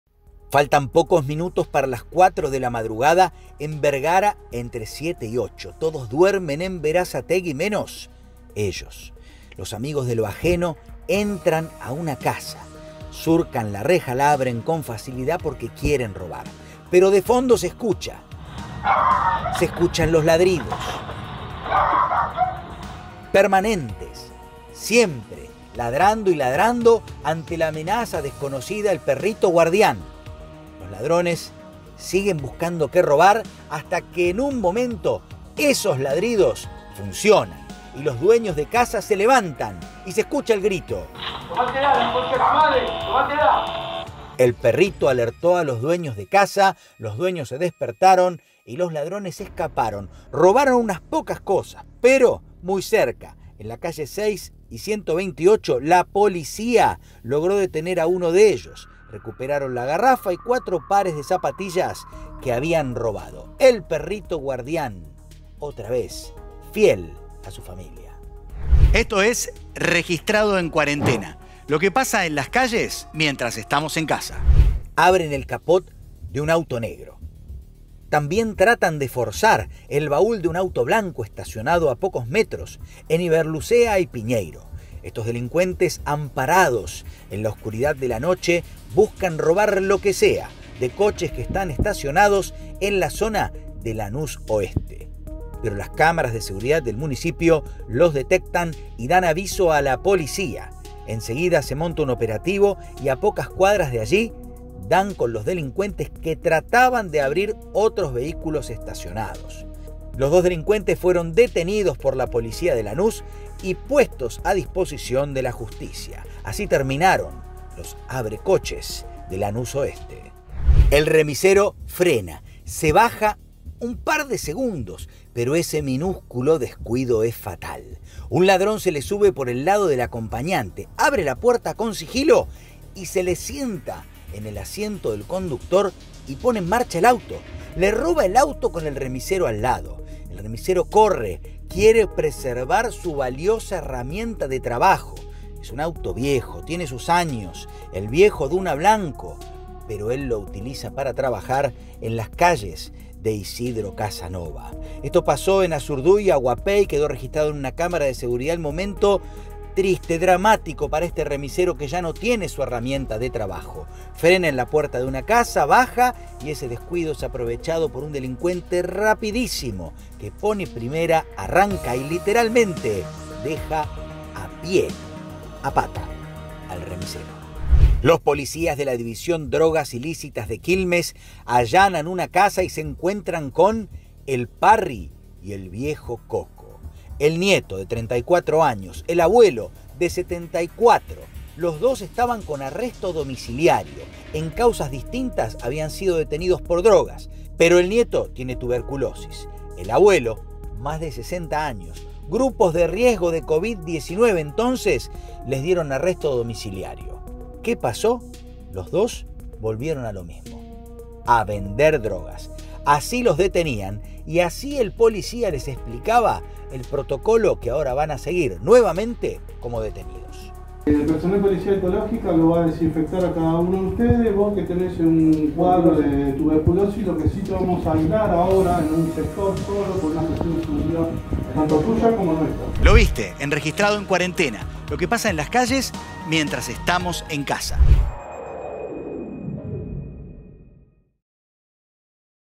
SALVADOS por su PERRITO GUARDIÁN - ABRECOCHES in fraganti - ATAQUE al REMISERO
Rec Registrado en cuarentena. Todo que pasa en las calles mientras estamos en casa.
🔴SALVADOS por su PERRITO GUARDIÁN
🔴ABRECOCHES in fraganti
🔴ATAQUE al REMISERO
🔴Abuelo y nieto TRANSAS